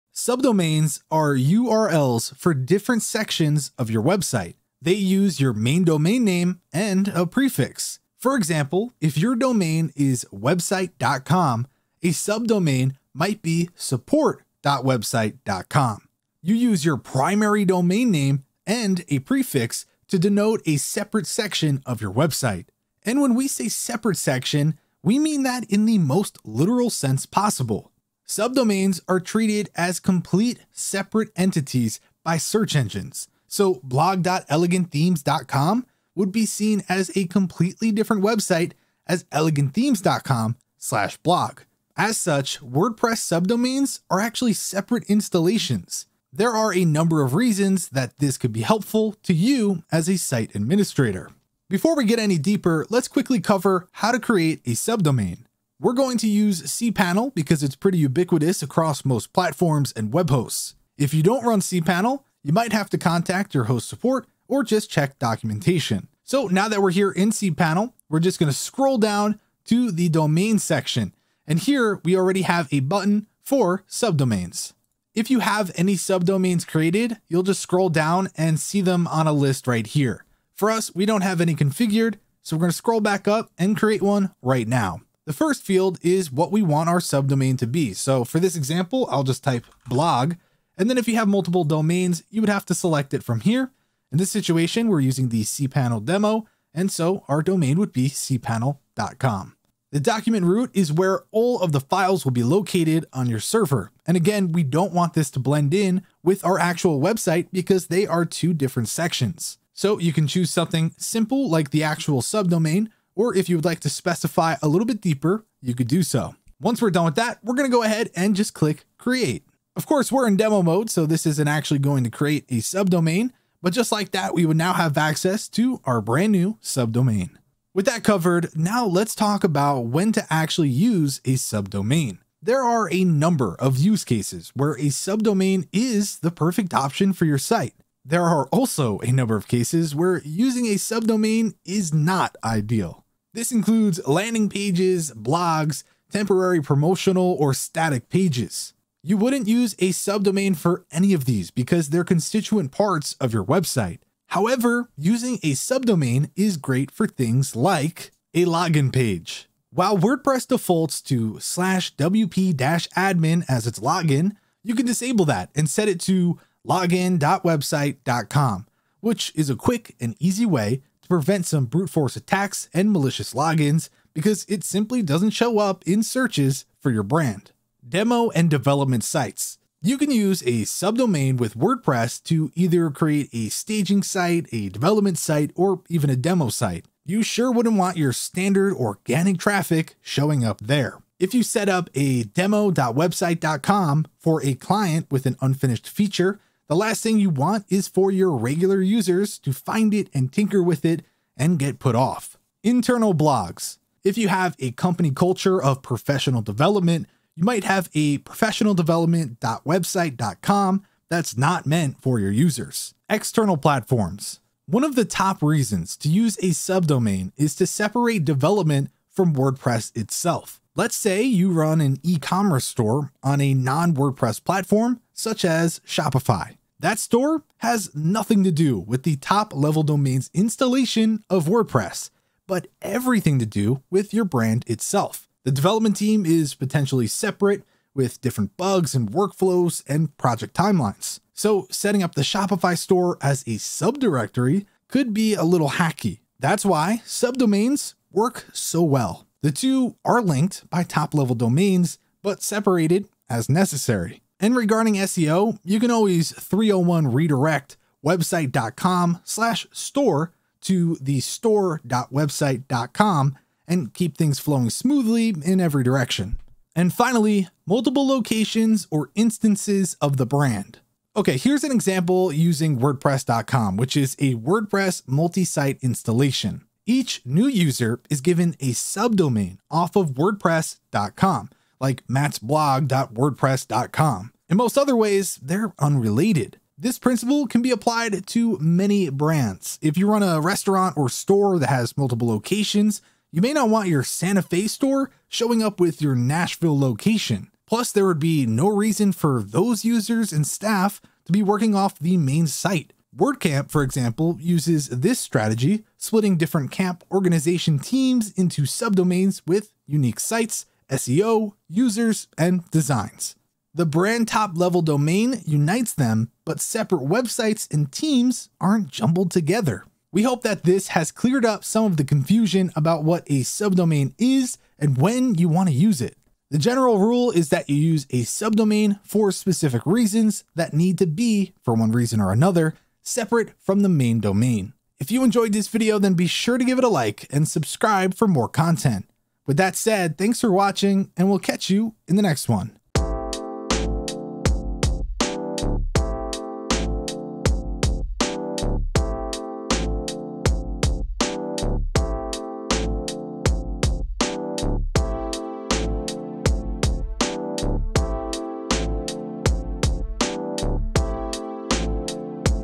What is a Subdomain and How to Use One with WordPress?
In today’s video, we're going to learn about subdomains.